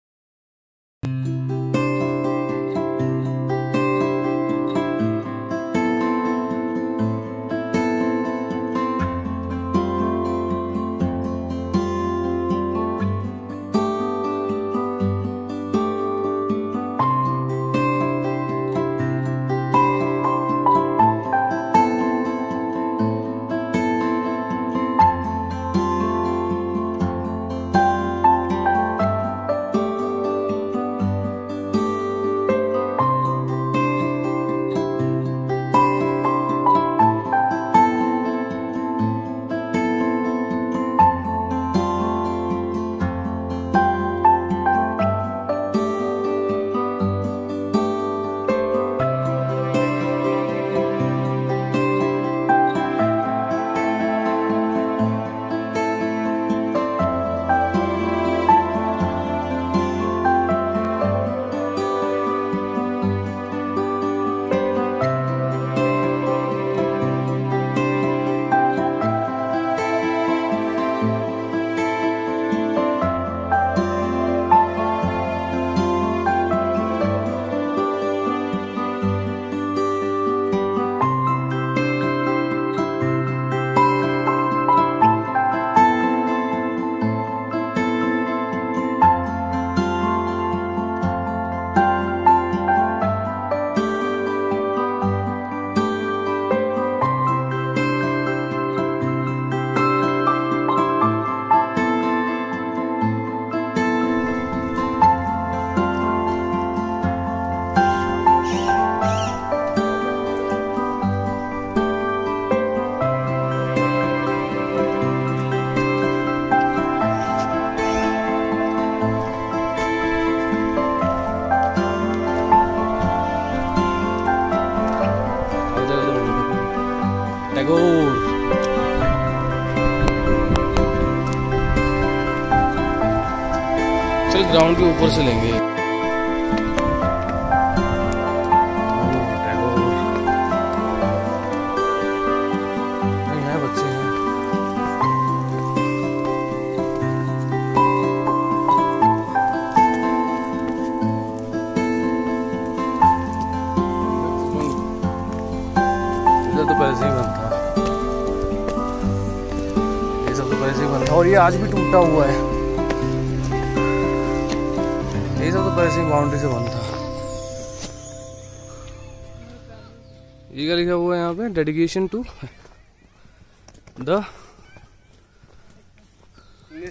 Army Public School DAGSHAI Part-1
I was in this school and i visited this school in november so i thought other people might be intrested to take a look at this video ,..